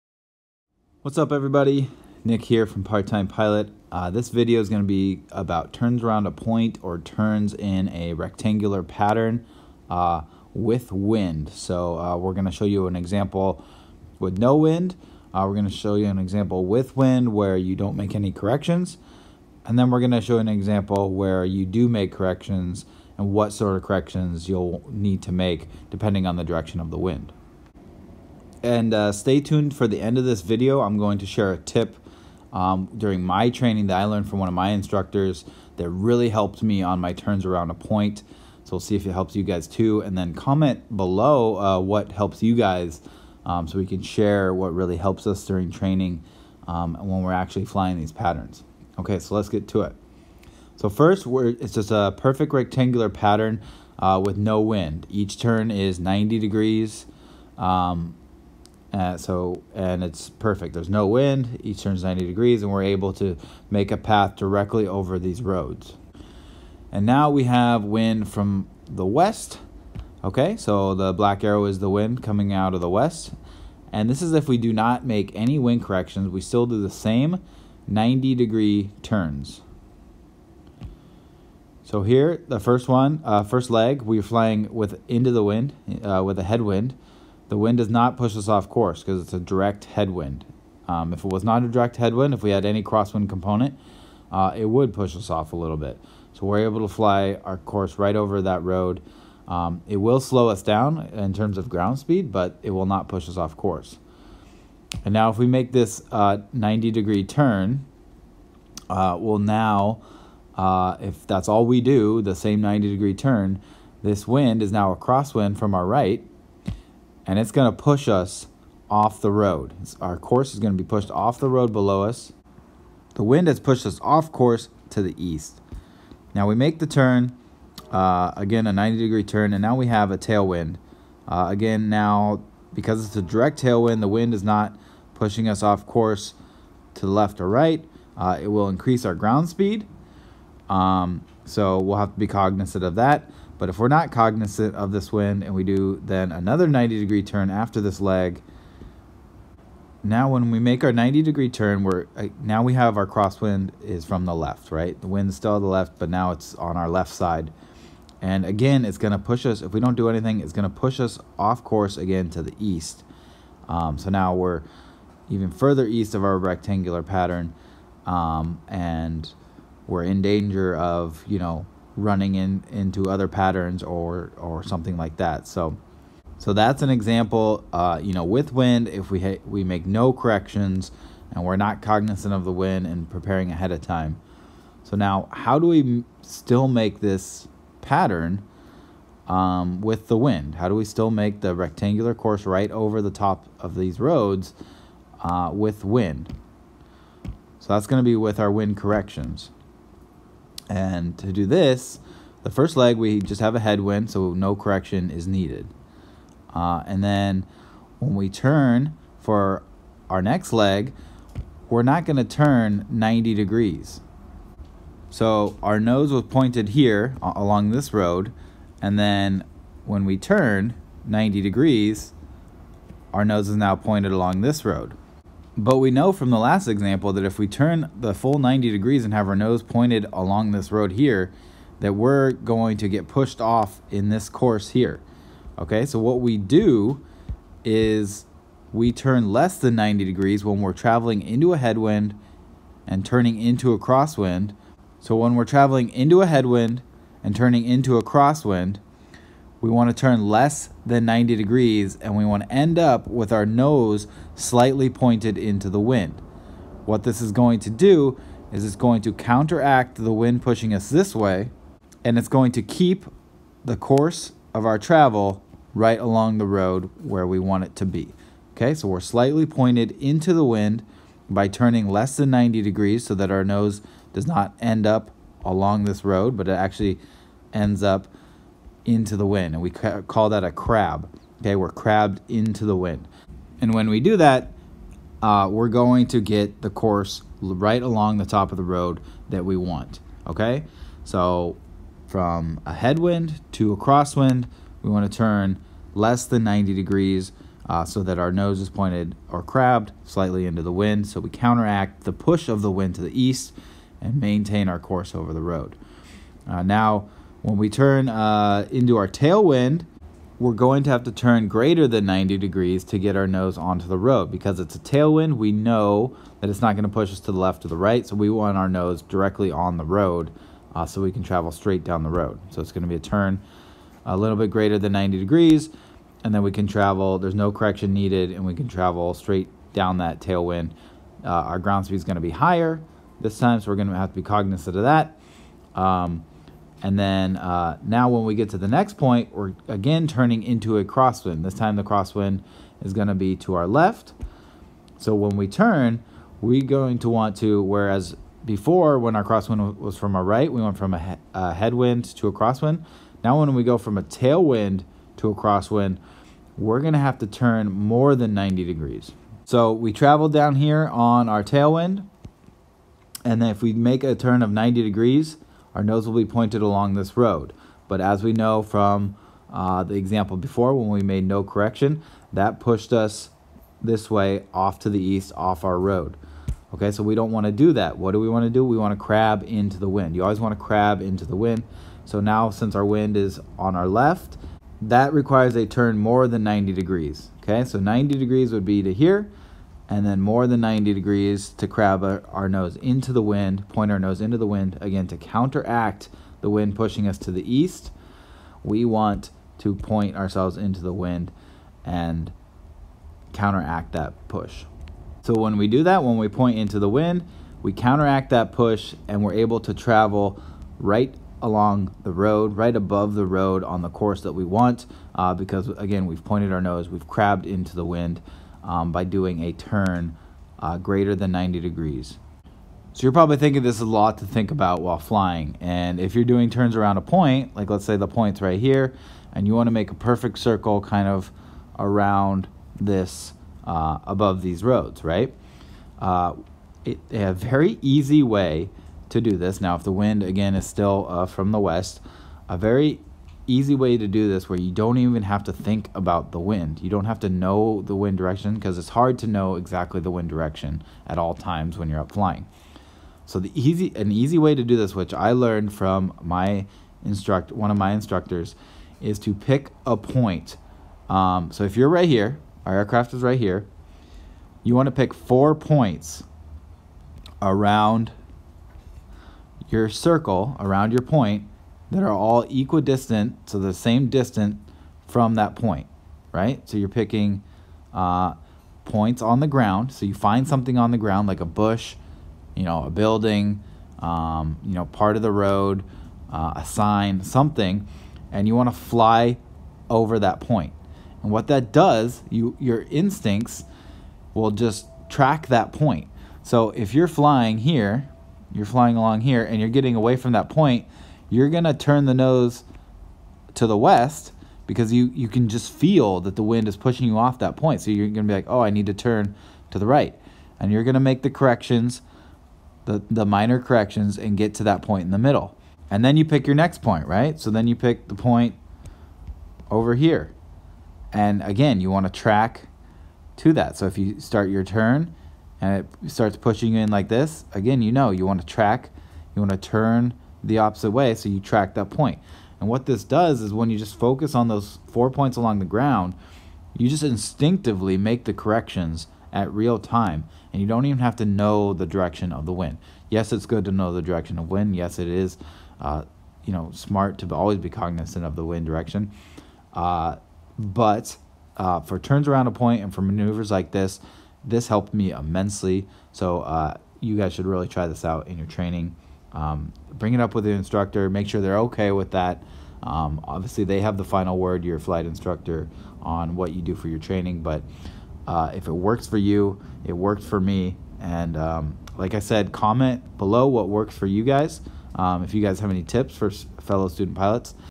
Turns Around a Point or Pattern with Wind Corrections - For Student Pilots
In this video, I go talk about the maneuver of turning around a point or making a rectangular traffic pattern. I go over examples with no wind, with wind but no wind corrections made, and finally an example with wind and the appropriate wind corrections made.

I also give a tip at the end of the video that helped me immensely during my private pilot training on the turns around a point maneuver.

🛫 Timestamps:
00:00 Intro
00:14 Turns around a Point - with Wind
01:01 Turns in a Rectangular Pattern - no wind
01:16 Wind from the West - No Correction
01:32 Flying with Headwind - Wind does NOT push us off course
01:55 Crosswind to our Right - Wind pushes us East off course
02:16 Flying with Tailwind - Wind does NOT push us off course
02:40 Crosswind to our Left - Wind pushes us East off course
03:15 Wind from the West - With Correction
03:30 Flying with the Headwind - No correction needed
03:38 Crosswind to our Right - Point nose into wind (Crab)
05:17 Flying with Tailwind - No correction needed
05:48 Crosswind to our Left  - Point nose into wind (Crab)